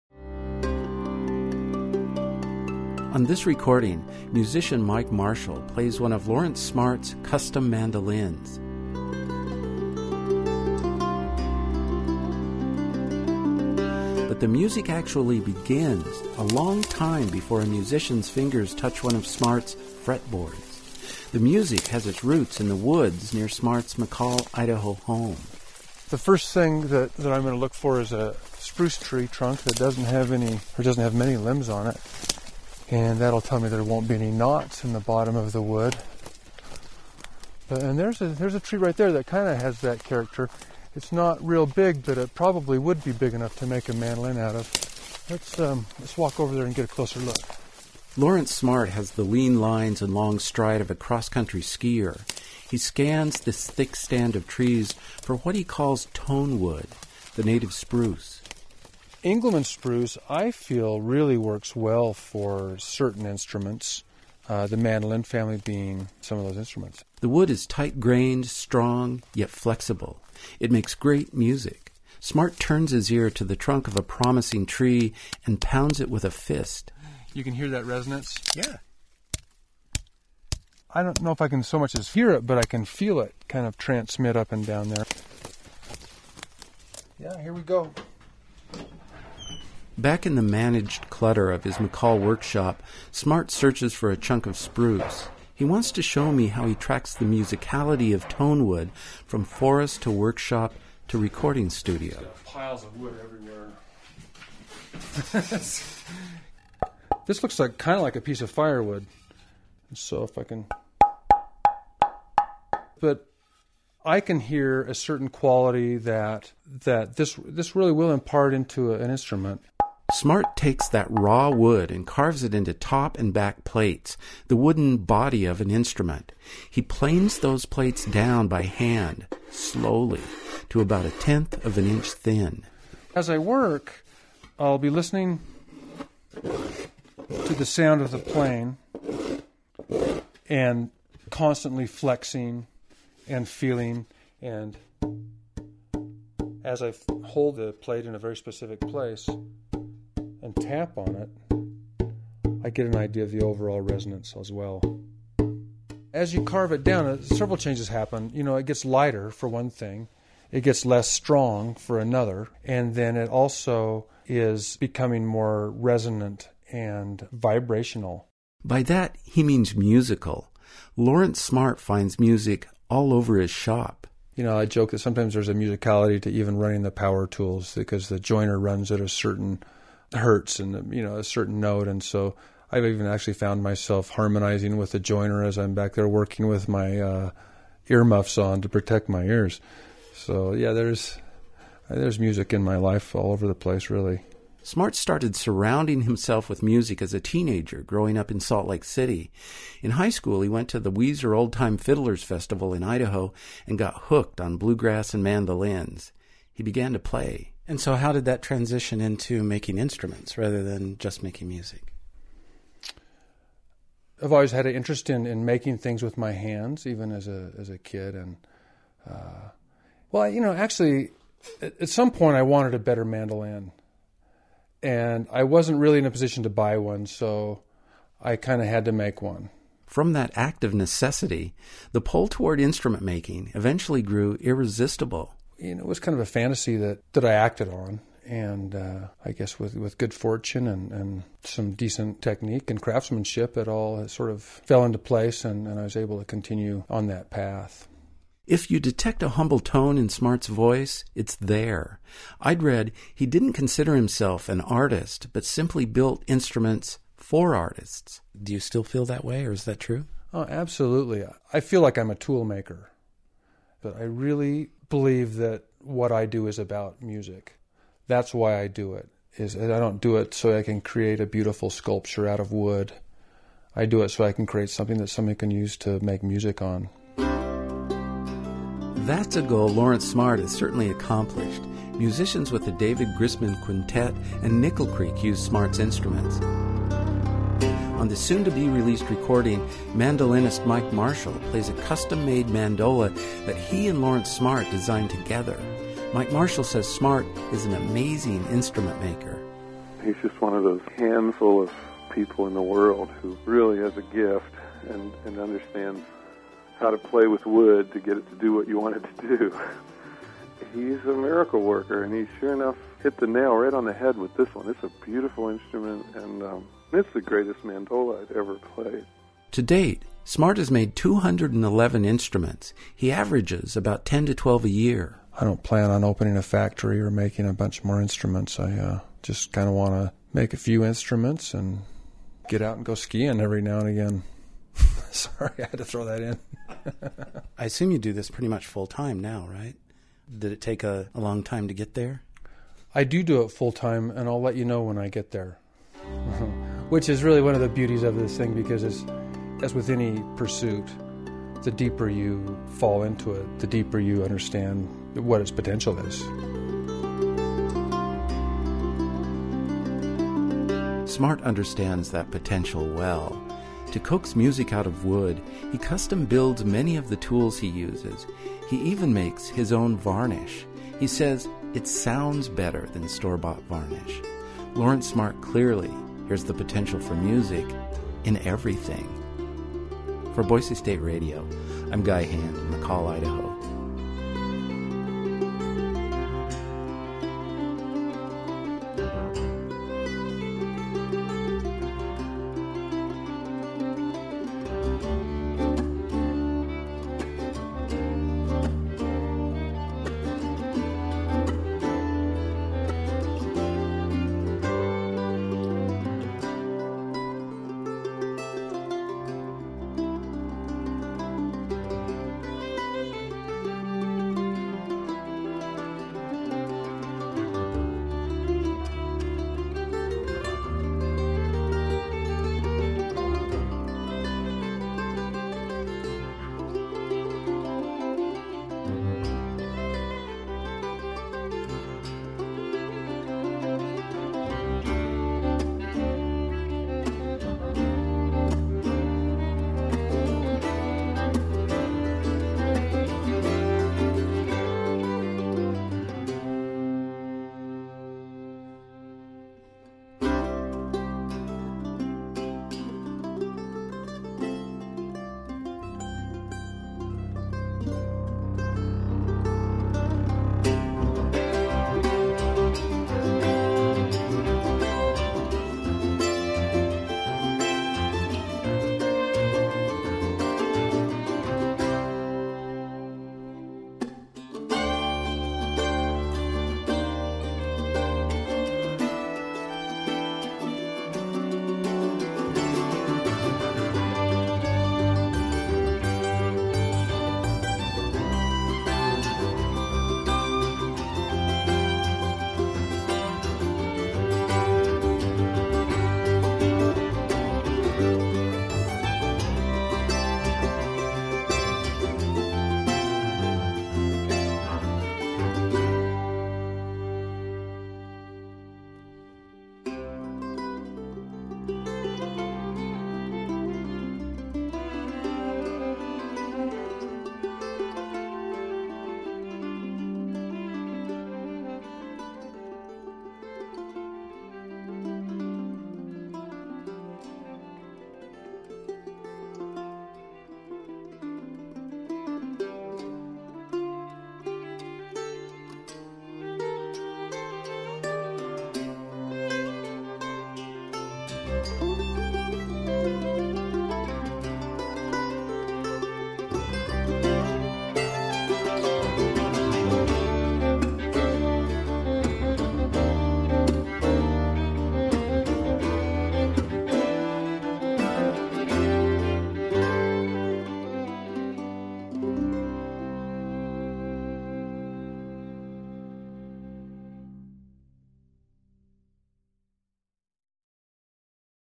A. Lawrence Smart Interview
This is an interview with renown luthier A. Lawrence Smart of Hailey, Idaho. In this BSU broadcast, Smart discusses his process for wood selection, and lutherie as a mechanism for creating tools. Also included in this interview is Mike Marshall who speaks about Smart's incredible talent as a woodworker, and about his commisioned Smart mandola, which he calls "The best mandola he has ever played."